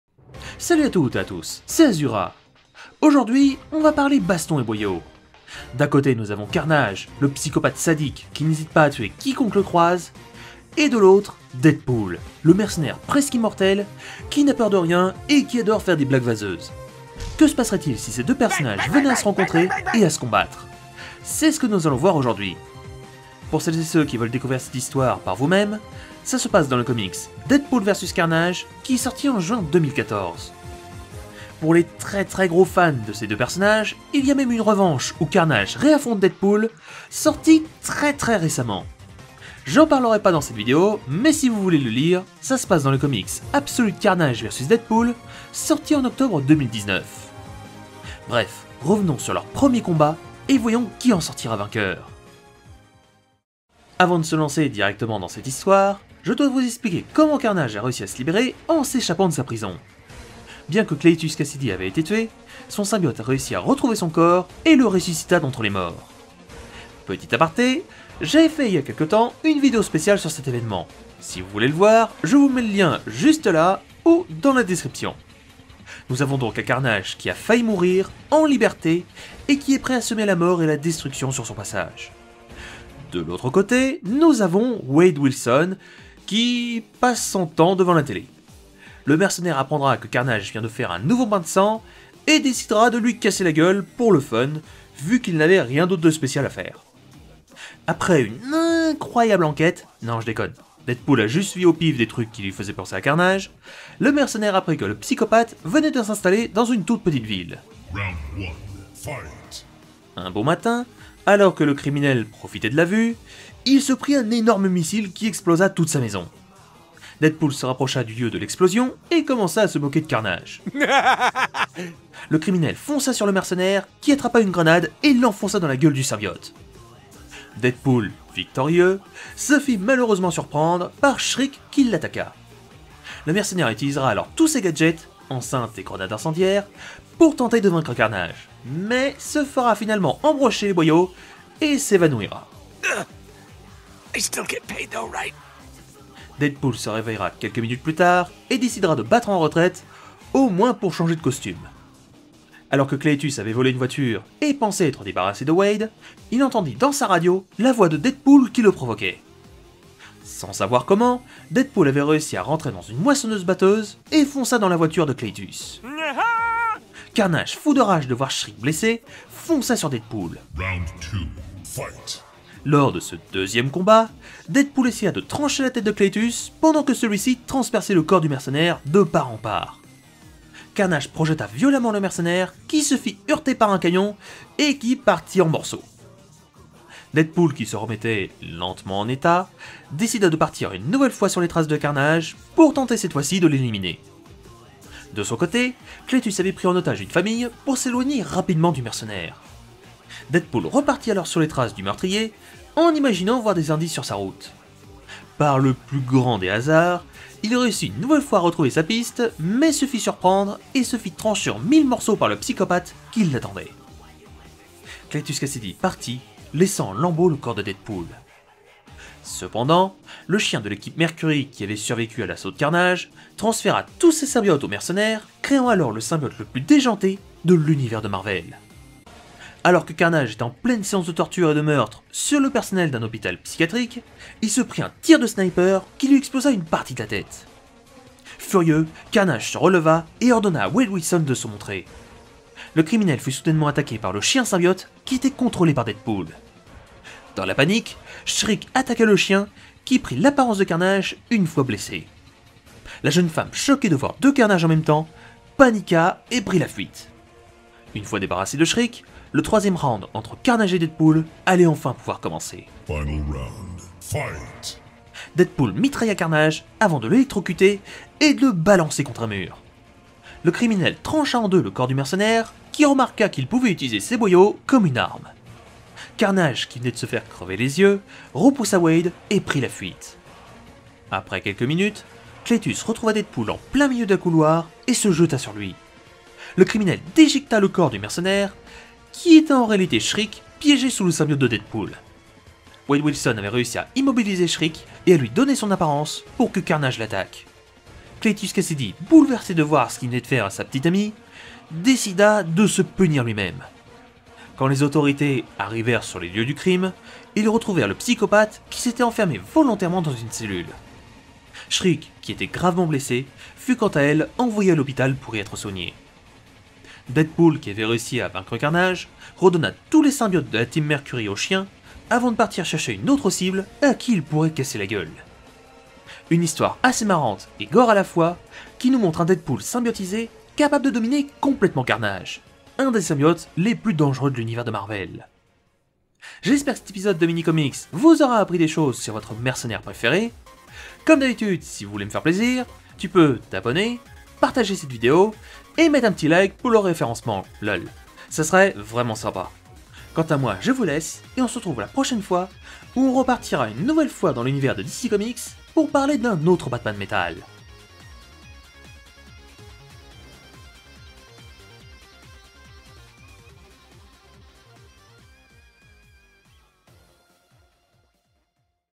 [Mini-Comics] Carnage vs Deadpool
Nouvel épisode de Mini-comics où on va parler de d'un combat entre 2 personnages très puissant de l'univers de Dc comics ! L'un est un symbiote psychopathe qui tue pour le plaisir l'autre est un mercenaire presque immortel qui adore faire des blagues ! On va parler du combat de Carnage vs Deadpool !

N’hésitez pas à me proposez des personnages que vous voulez voir ou si vous avez des idées pour des Mini-comics ! activez la 🔔 pour ne rien louper !!